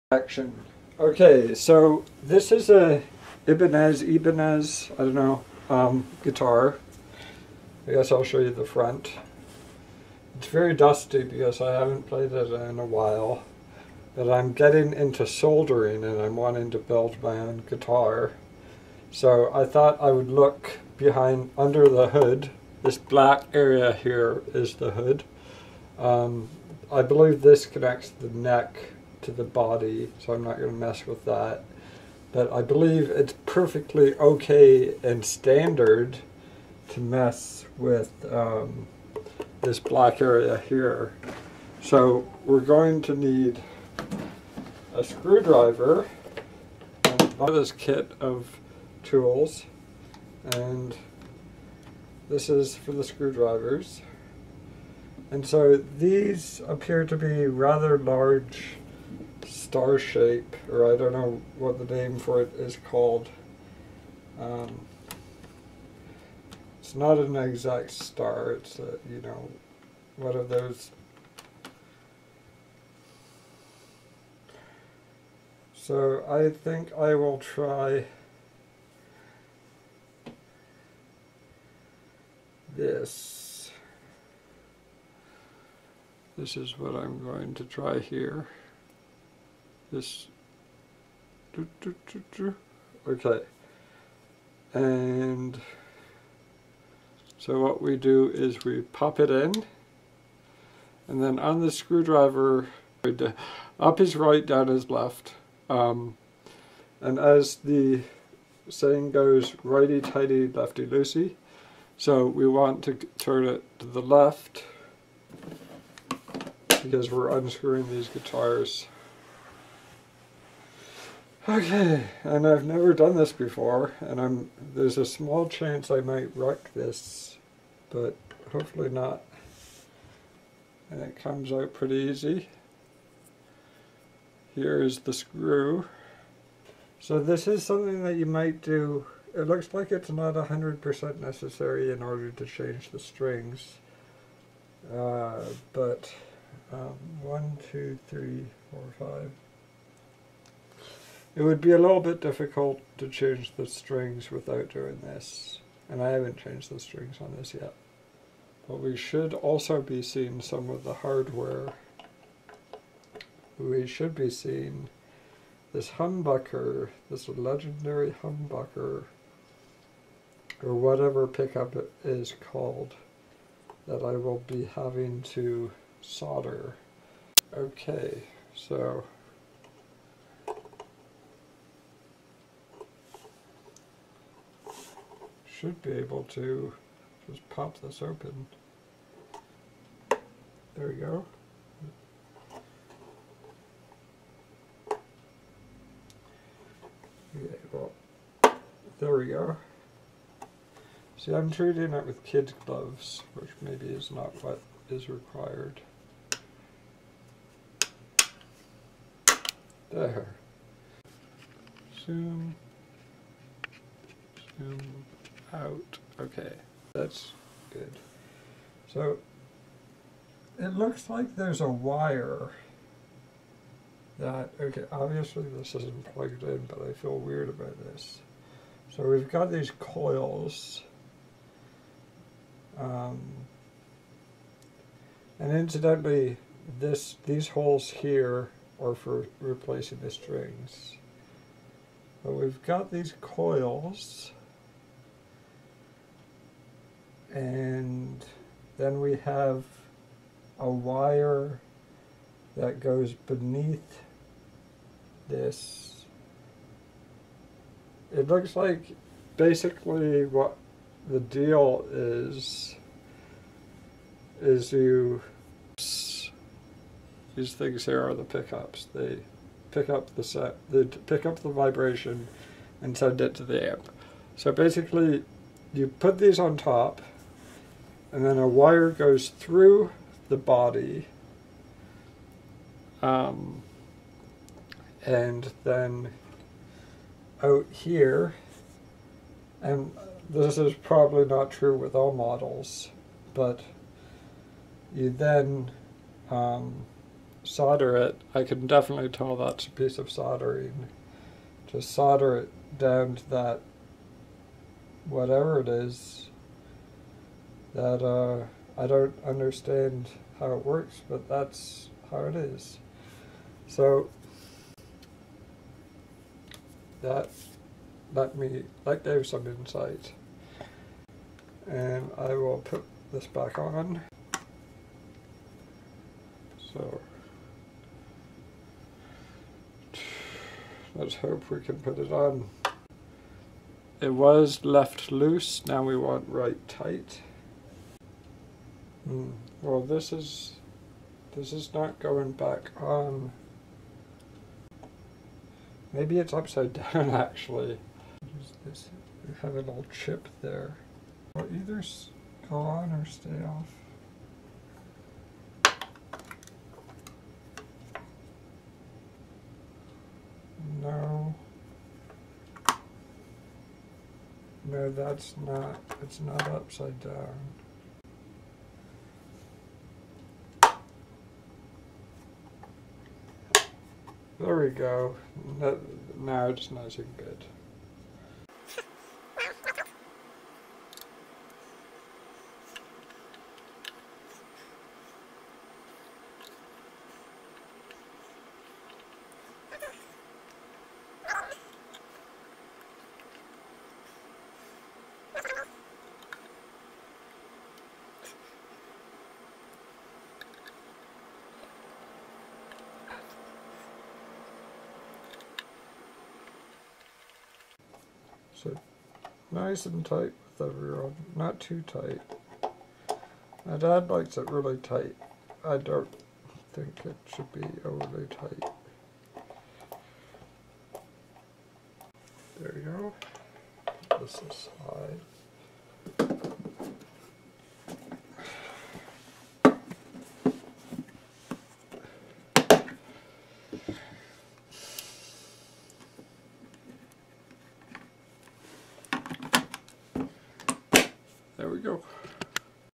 Looking at the inside of an Ibanez Gio Electric Guitar
I opened up the back of the guitar to look at the components inside